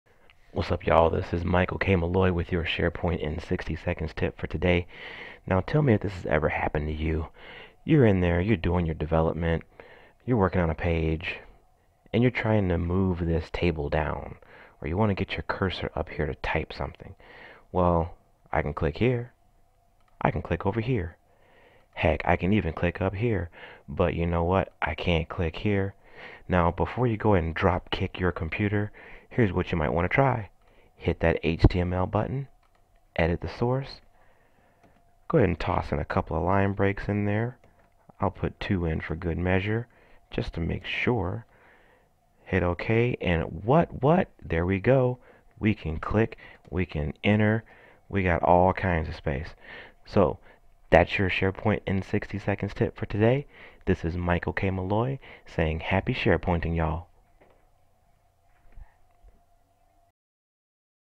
How To Place The Cursor Above A Table In SharePoint
This SharePoint tutorial will demonstrate a quick and easy way to get your cursor above a table. Normally, this isn't an issue. However, when you have a table that is at the very top of a page or zone then it is nearly impossible to get your cursor above the table. By adding a few line breaks in the HTML, you are able to free up a little space above the table so you can continue developing and refrain from drop kicking your computer!